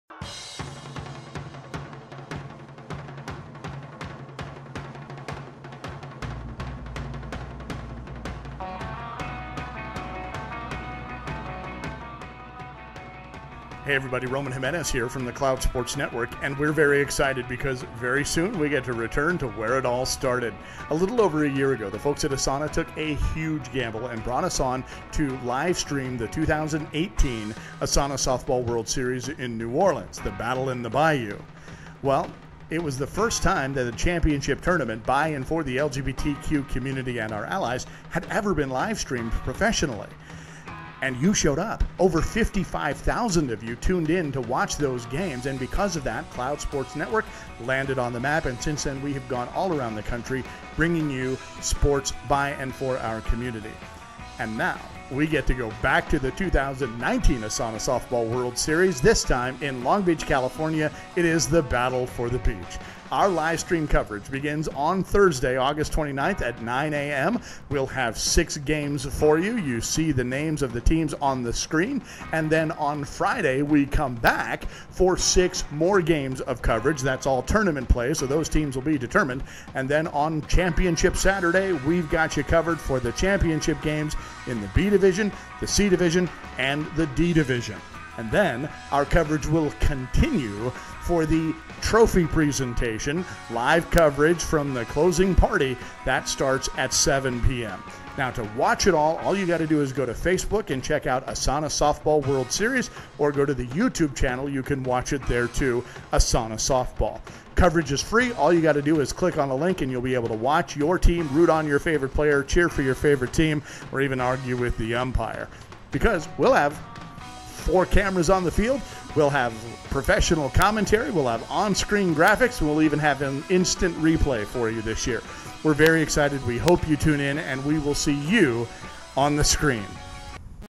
2019 ASANA Softball World Series "Battle For The Beach"
Coming to a screen near you... the 2019 ASANA Softball World Series in Long Beach, California, the Battle for the Beach!

Join over 1,500 athletes, coaches and fans from 25 member cities across North America at this year's Series. If you can't be there in person, then be there online and join over 50,000 other viewers who will be live-streaming the coverage on Facebook and YouTube!

To watch the coverage live on Facebook, click  this link for the ASANA Softball World Series Facebook page:

To watch the coverage live on YouTube, click this link for the ASANA Softball World Series YouTube channel: